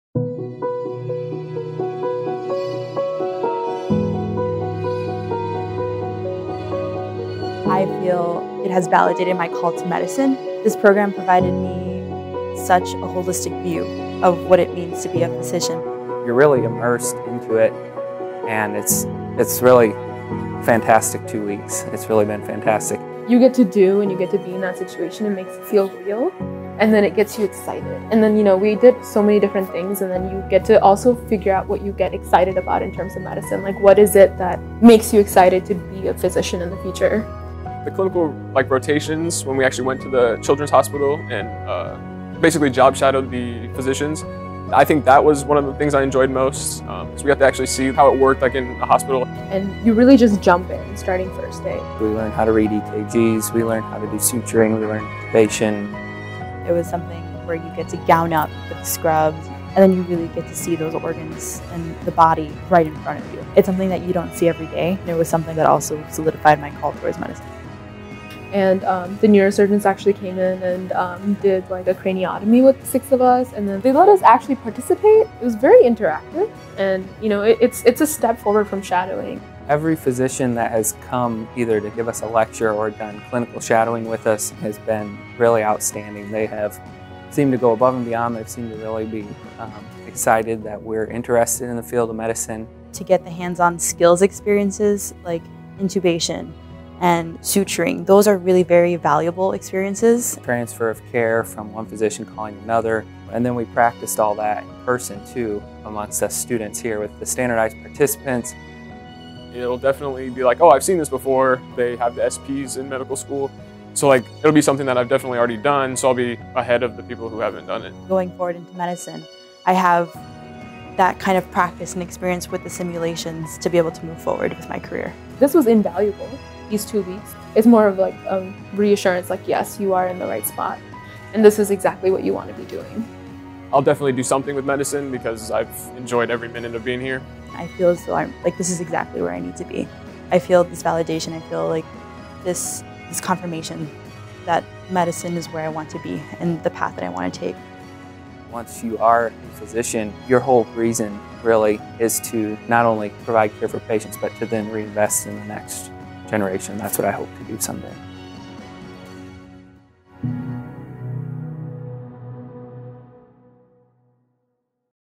Summer Pre-Med Immersion Experience
The Jump Simulation Pre-Med Immersion Experience is an immersive, two-week clinical experience for college students planning to attend medical school. Participants of this program are exposed to a range of clinical roles within OSF HealthCare and get a taste of the medical school experience. Competitive scholarships are available.